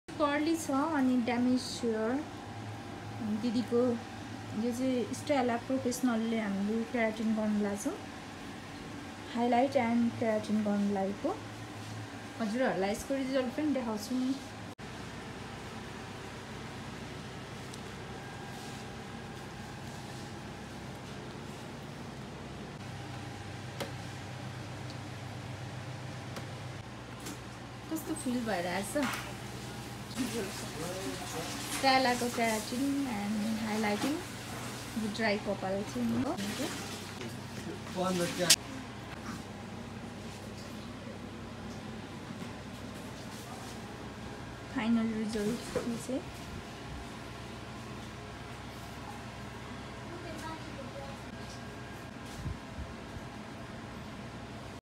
Hair keratin Stella professional the best parlourkhichapokhari newroad Kathmandu content 9816141388
Stella professional dry damage hair for
Before hair highlighting with hair keratin
If you want to like visit our the best parlourkhichapokhari location sundhara newroad mahalaxmi complex 2n 3rd floor Kathmandu content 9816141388🙏🙏